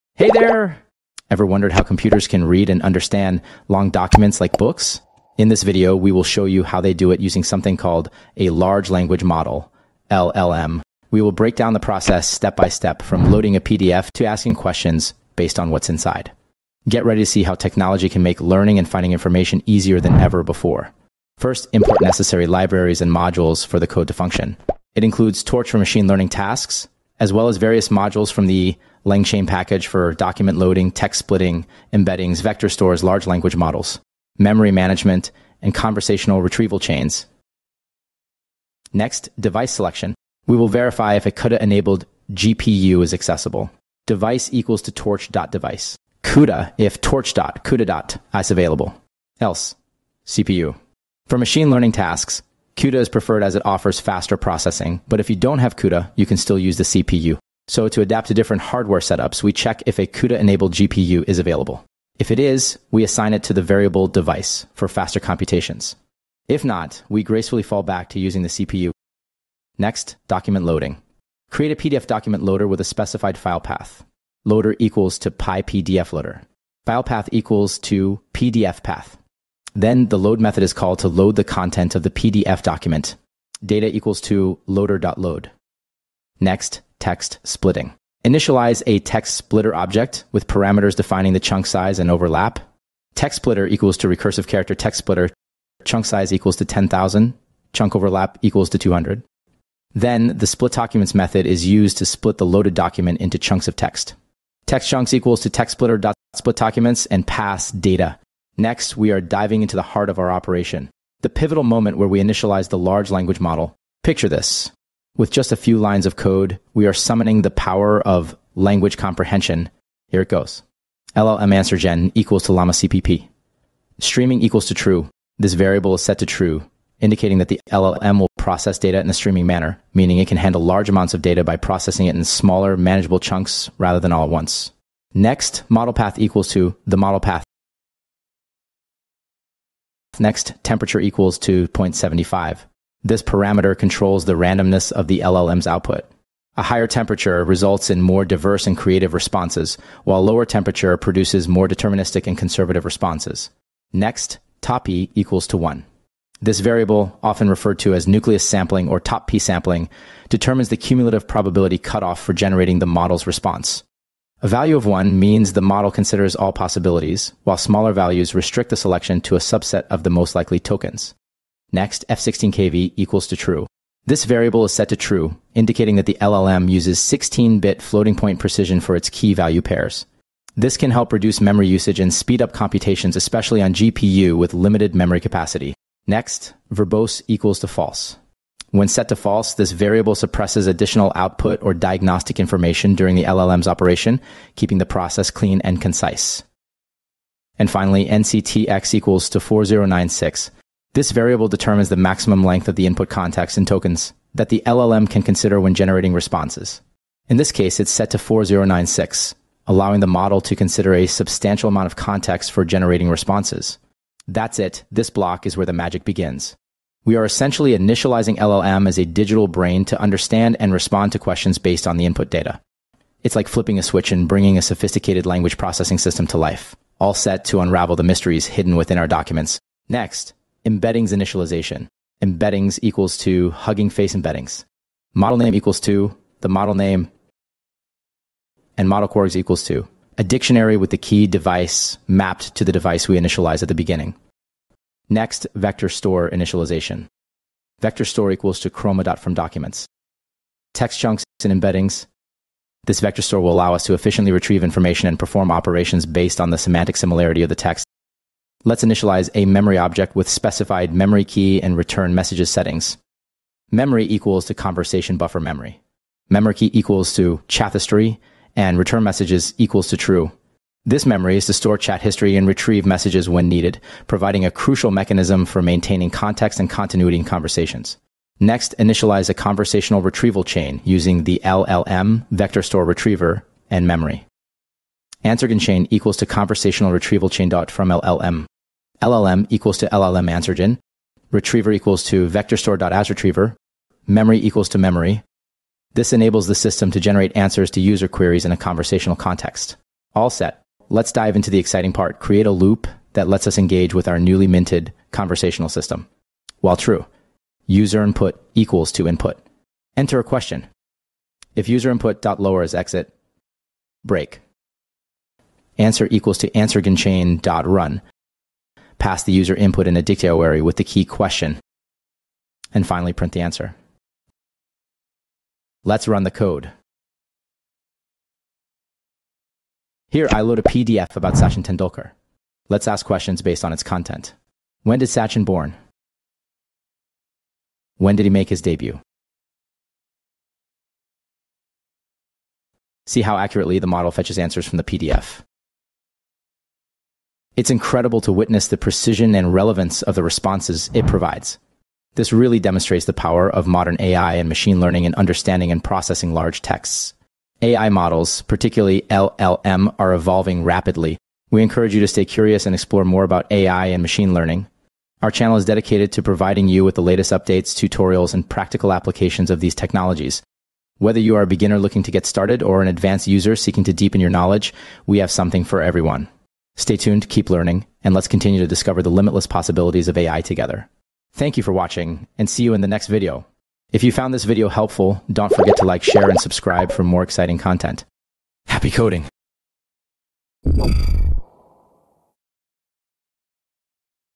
Chat with PDF using langchain without OpenAI Key
Langchain Question Answering . "Build a ChatGPT-Powered PDF Assistant using langchain and PyTorch | Step-by-Step Tutorial"

In this comprehensive tutorial, you'll embark on a project-based journey where we leverage LangChain and PyTorch to develop an interactive assistant for your PDF documents. Using the power of an Advanced Large Language Model (LLM), we'll create an application that enables you to ask questions about PDFs and receive accurate answers. You'll learn how to load and process PDFs, split text into manageable chunks, generate embeddings, and build a conversational retrieval system. This hands-on guide will equip you with the skills to harness the capabilities of modern AI for efficient document analysis and interaction.

This Chat with PDF langchain project demonstrates how to utilize a Large Language Model (LLM) to process and understand PDF documents for question-answering tasks.
The implementation leverages several powerful libraries to achieve this:
Torch: For leveraging GPU acceleration in machine learning tasks.
Langchain: A suite of tools for loading documents, splitting text, generating embeddings, and more.
HuggingFace Embeddings: For creating semantic representations of text.
Chroma: For building a vector store that enables efficient similarity-based retrieval.
LlamaCpp: An LLM used for generating answers based on the processed text.
Conversational Retrieval Chain: For maintaining context and generating accurate responses during interaction.

Key Features
Device Adaptation: Automatically uses CUDA-enabled GPU if available, otherwise falls back to CPU.
Document Loading: Loads and processes PDF documents to prepare for analysis.
Text Splitting: Efficiently splits large texts into manageable chunks.
Embeddings and Vector Store: Utilizes HuggingFace embeddings and Chroma vector store for efficient information retrieval.
Conversational Interface: Engages users in a Q&A session using the LLM with context-aware responses.

Python Code Camp😊